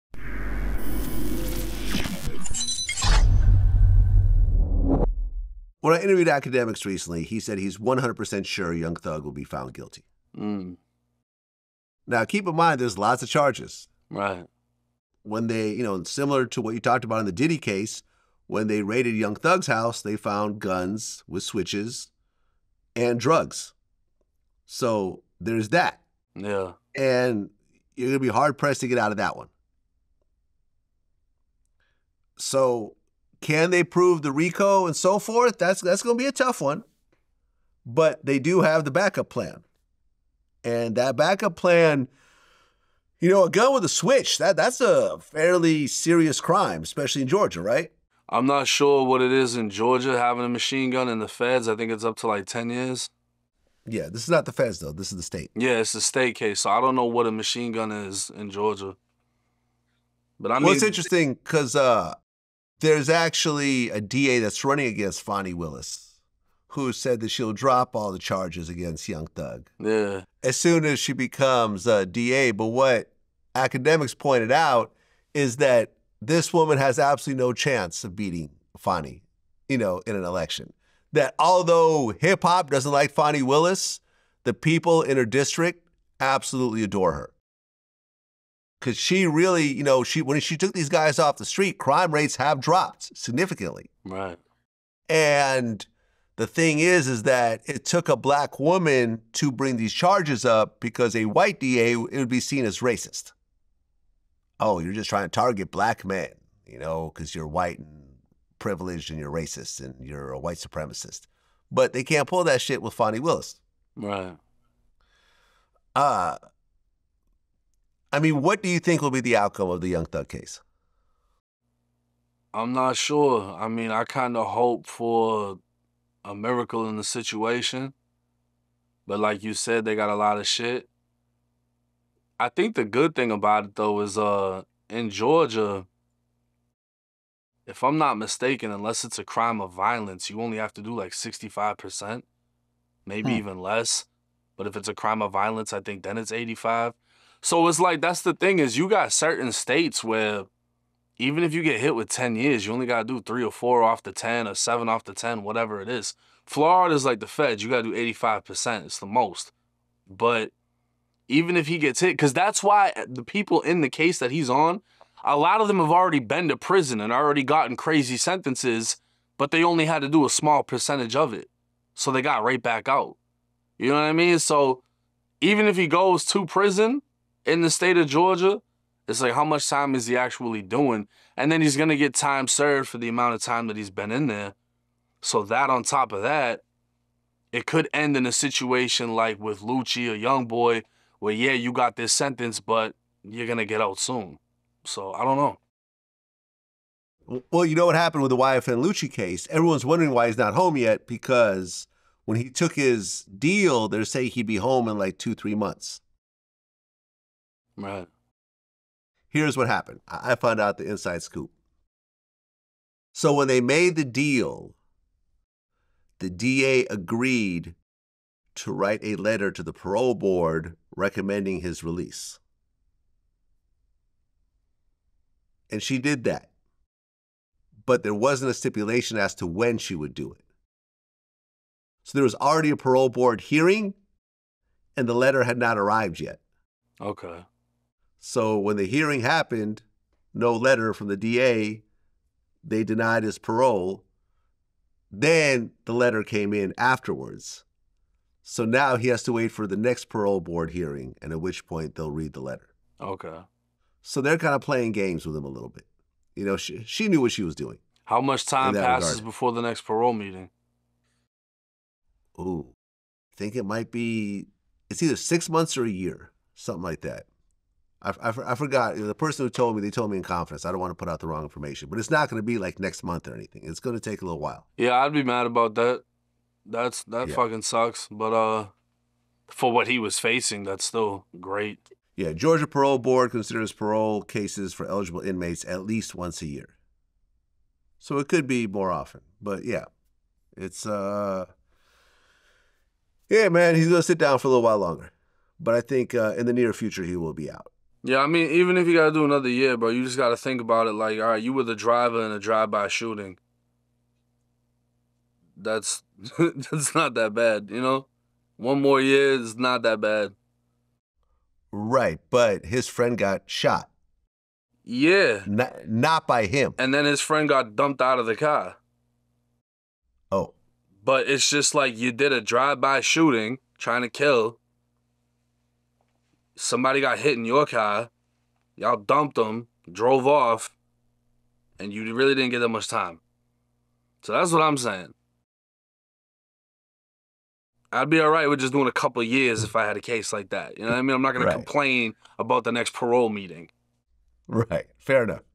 1090 Jake Breaks Down Why Young Thug May Not Get Much Time if Found Guilty (Part 7)
1090 Jake provided insights on why rapper Young Thug might not face extensive jail time even if found guilty of the charges against him. Vlad recounted how some experts are convinced of Thug's guilt, mentioning the serious nature of the charges, such as gun possession with switches and drugs found during a raid. However, 1090 Jake emphasized the differences between federal and state penalties, noting that Georgia state laws may be more lenient. He explained that in Georgia, non-violent offenders typically serve around 65% of their sentence, as opposed to the federal requirement of 85%. Additionally, there’s a political angle to consider: a district attorney candidate opposing Fani Willis has vowed to drop all charges against Young Thug if elected, although her chances of winning are slim. Despite what seems like a daunting legal battle, if convicted, Young Thug could benefit from time already served and end up spending less time behind bars than anticipated.